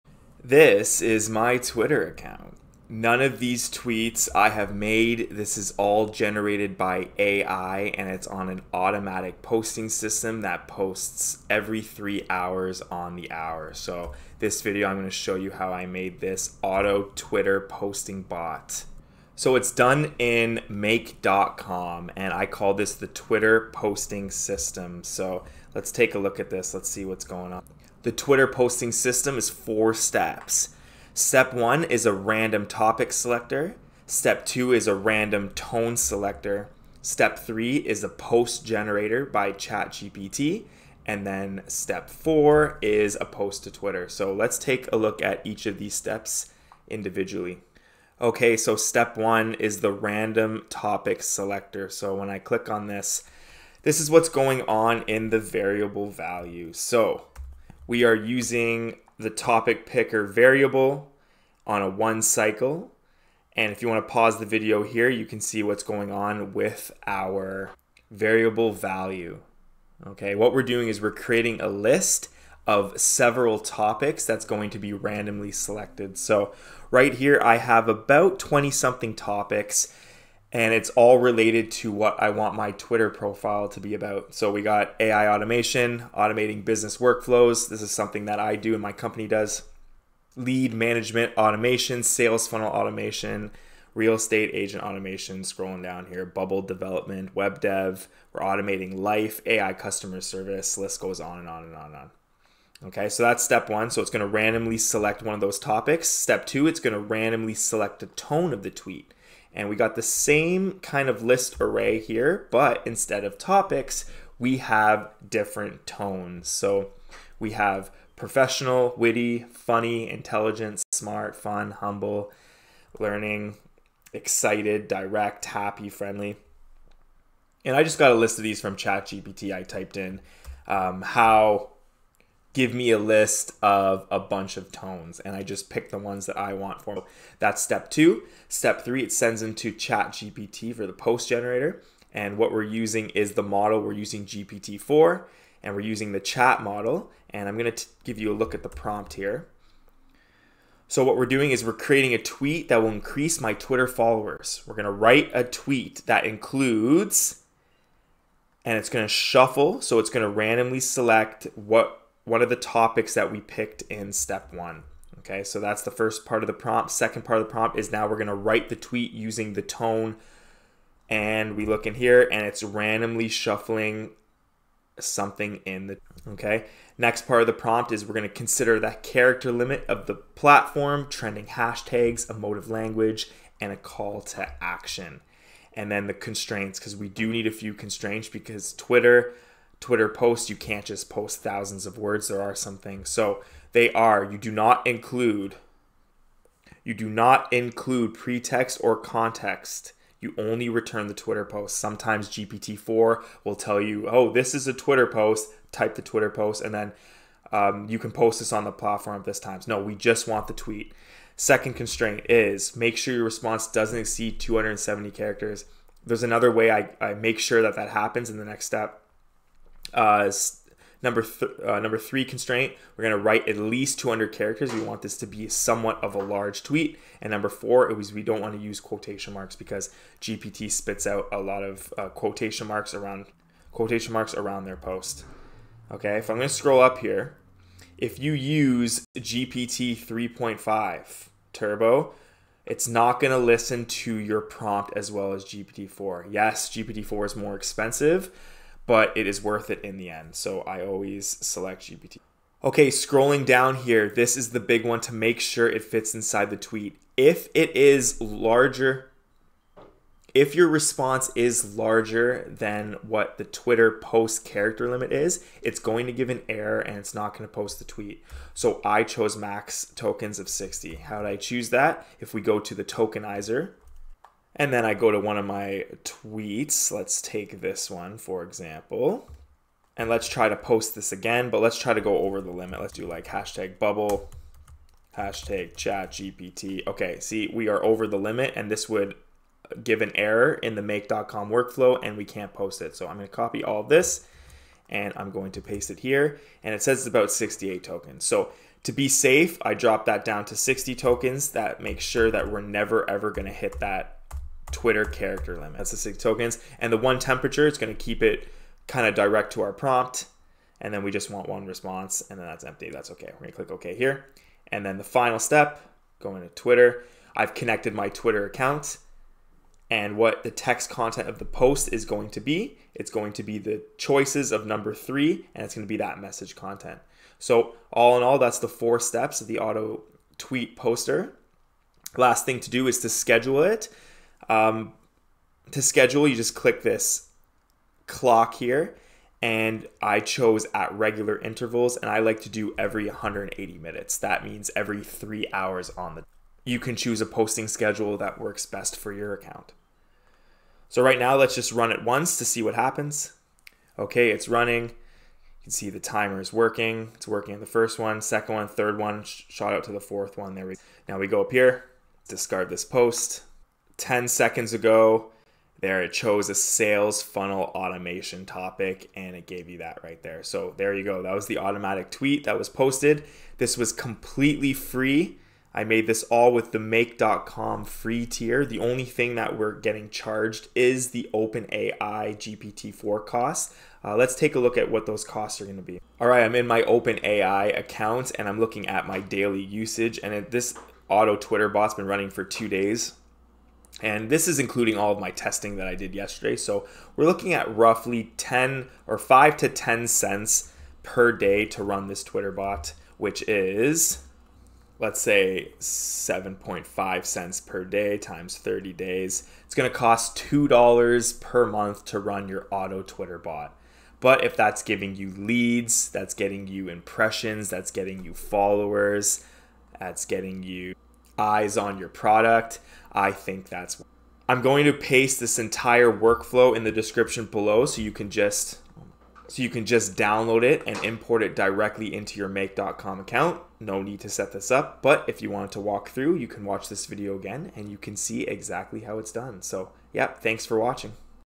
AUTO-POST to Twitter with an AI Agent | Grow Your AUDIENCE!
I show you how to set up an automatic AI agent that auto-posts to your Twitter account. It randomly selects a topic, a tone, generates a Tweet using ChatGPT, and then auto posts it to Twitter. All of this is made using Make.com. It runs every 3 hours around the clock without me having to intervene. It's a great way to grow your Twitter audience, Tweet impressions, and exposure.